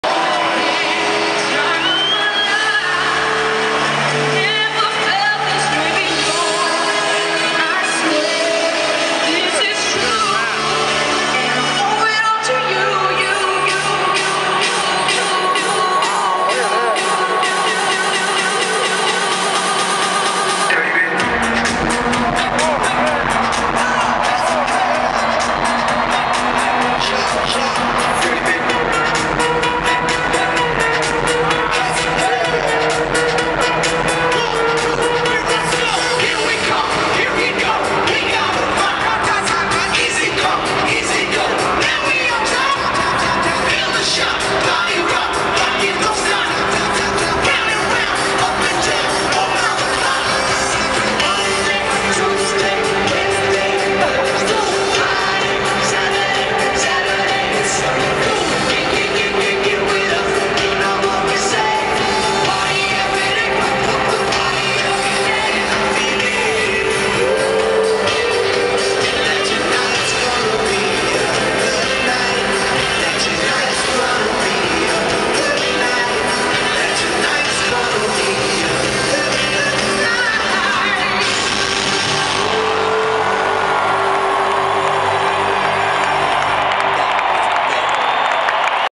Superbowl Halftime... Black Eye Peas (usher) Part 2
Superbowl 45 halftime show with all the gang and now usher